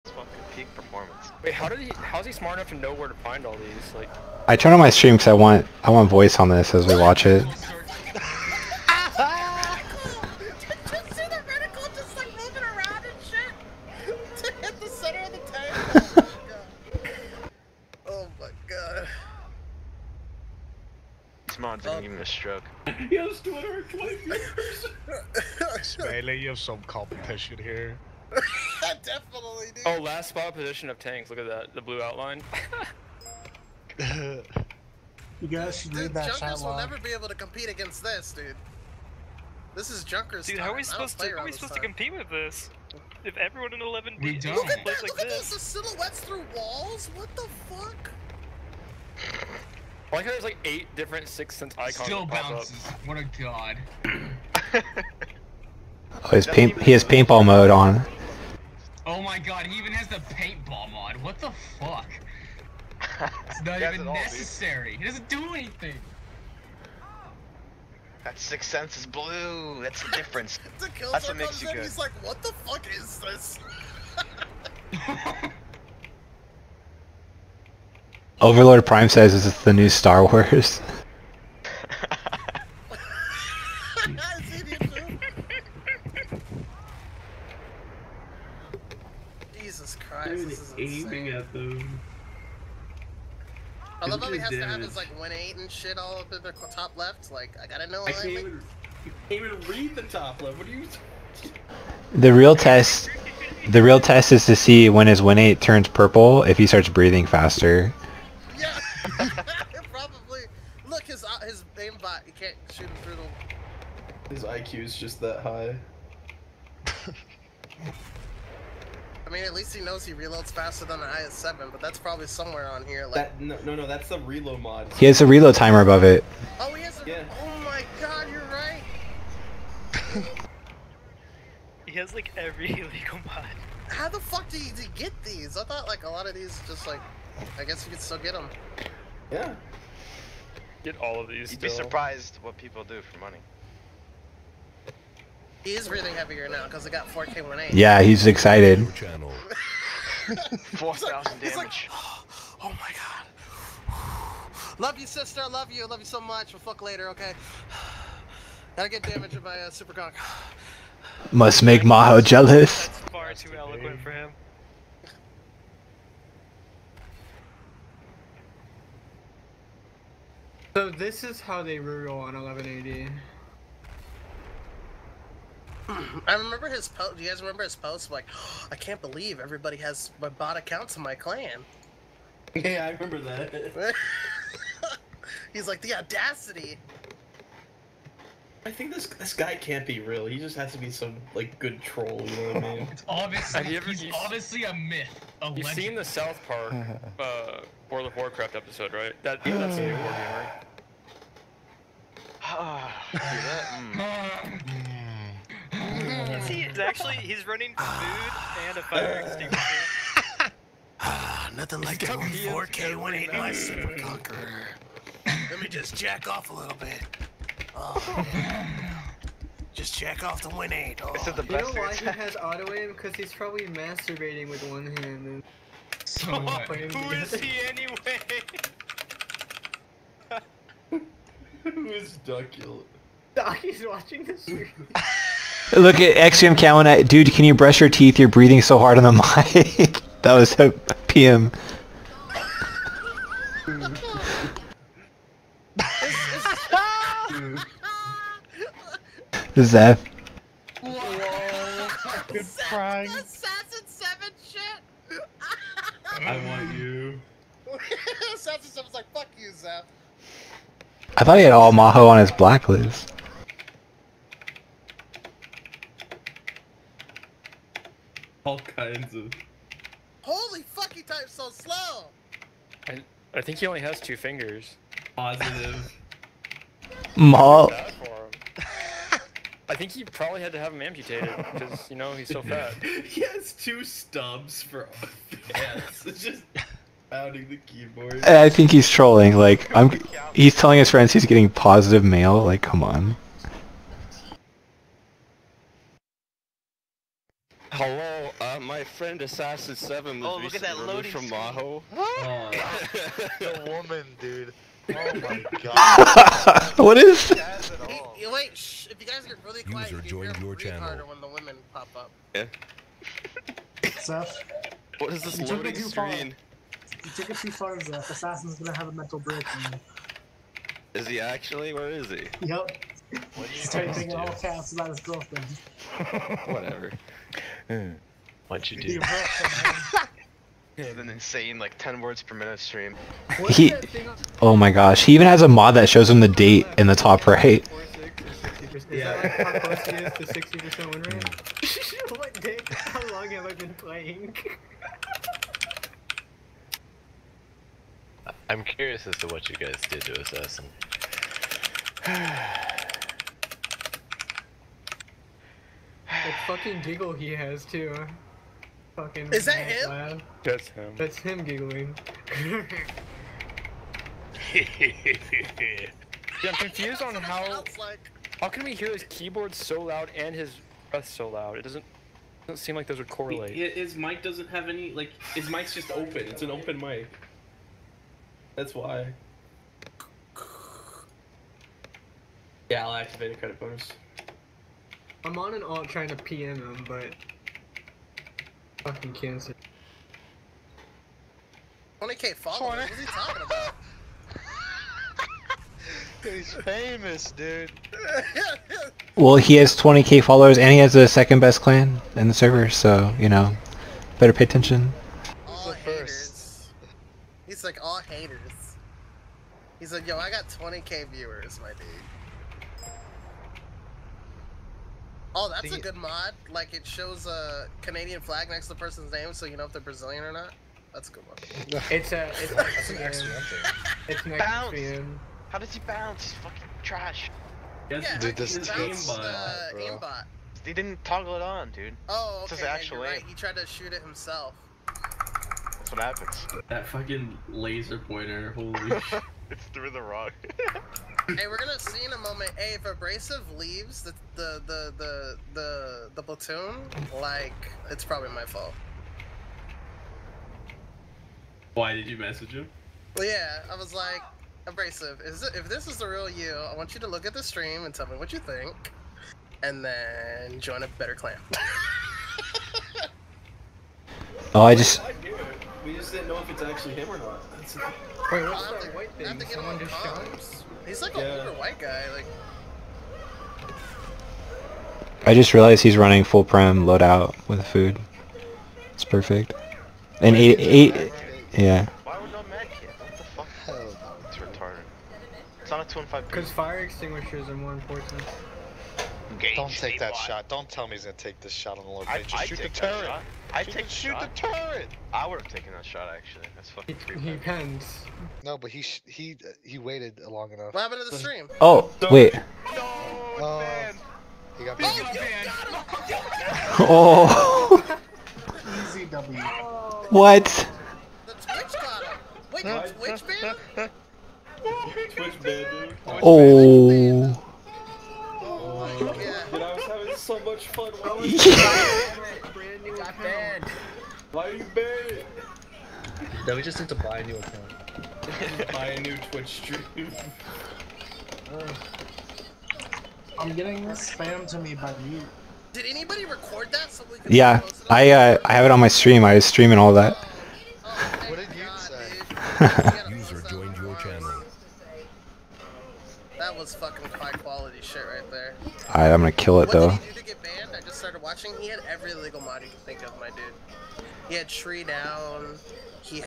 Noobwargamer Banned on Twitch Reaction Video
Reaction video of mahou comm's with noobwargamer getting banned. Sorry about the background cartoon, lol. Kids aren't in bed yet. Normally I don't record when they are up but this had to happen!